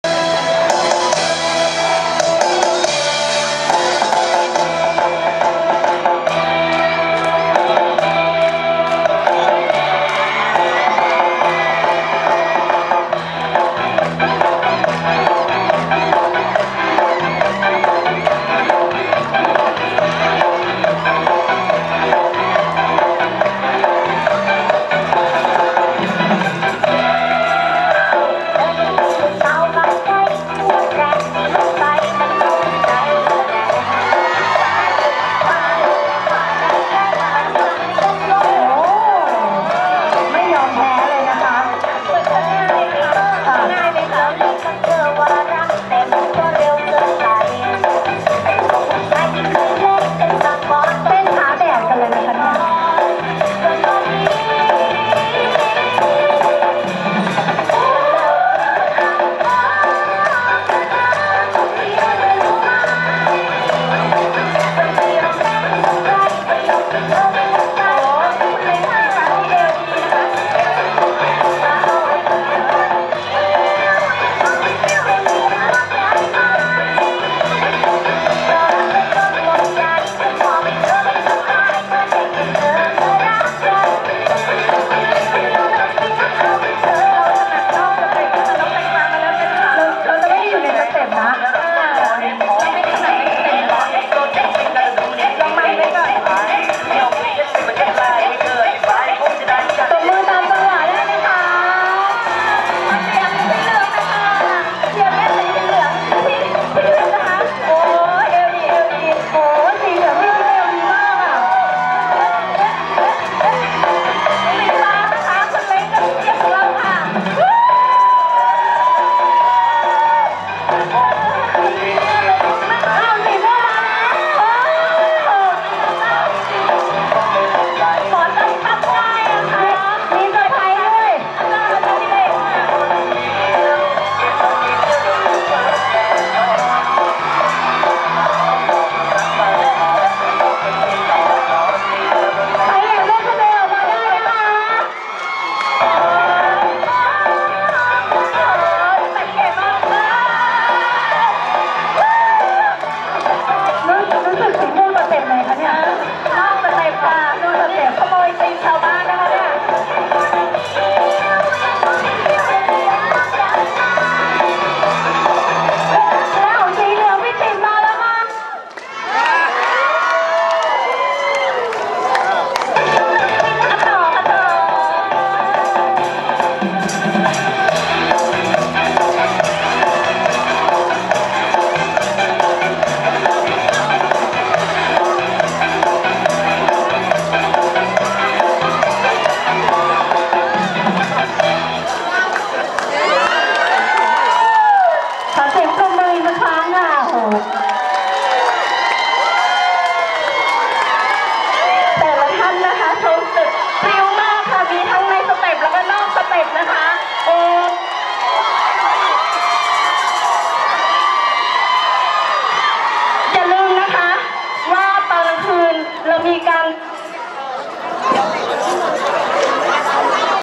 เต้นบาสโลบ งานกีฬาสี ภายใน โรงพยาบาลพระนั่งเกล้า 2562
โรงพยาบาลพระนั่งเกล้าจัดกิจกรรม งานกีฬาสีภายใน ลูกเจ้าสัวเกมส์ 2019 ณ ลานจอดรถ หน้าศาลพระพรหม ในวันที่ 20 ธันวาคม 2562 และงาน Sport  night